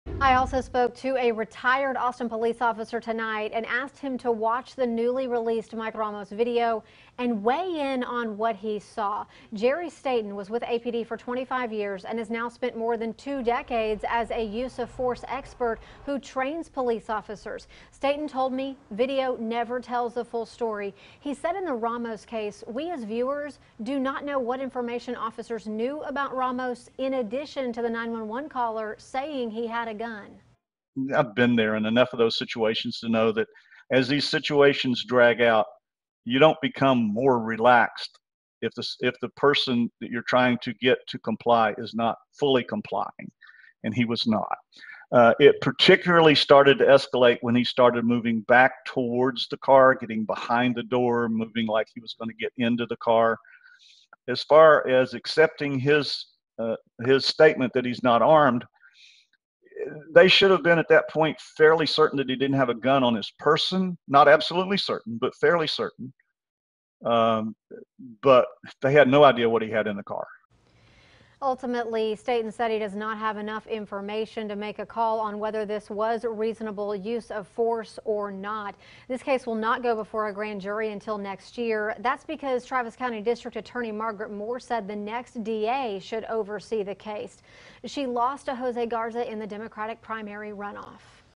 Former APD officer, use-of-force expert weighs in on Mike Ramos video
Jerry Staten, a former APD officer and now a law enforcement trainer on use of force spoke with KXAN's Erin Cargile about what he saw on the video of the Mike Ramos incident